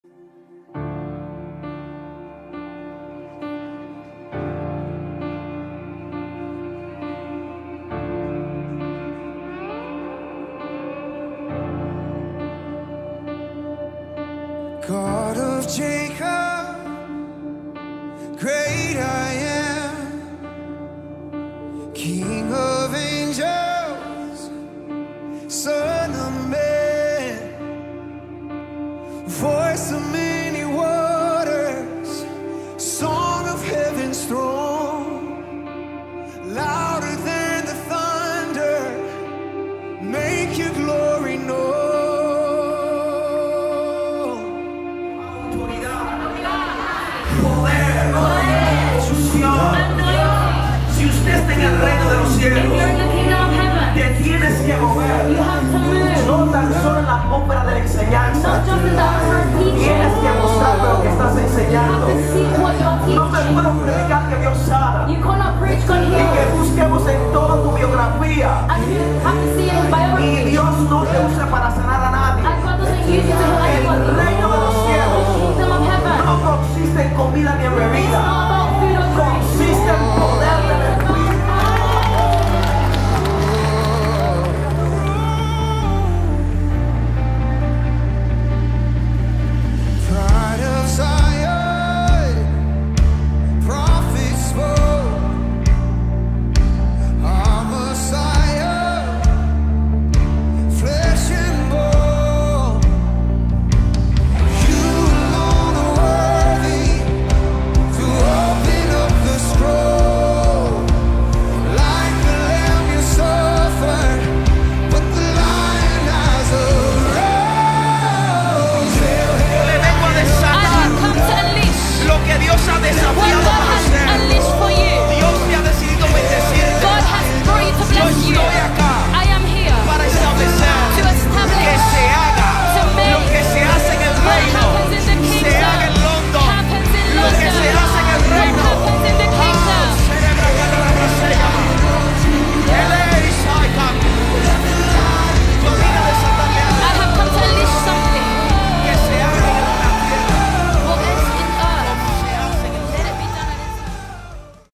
Trailer Cruzada con el Profeta David Linares en Londres | La Gloria del Shaddai
Profeta David Linares. Fue un momento maravilloso, lleno de profecías y avivamiento a través del Profeta y los Pastores. Es una verdadera bendición contar con estos hombres y mujeres de Dios.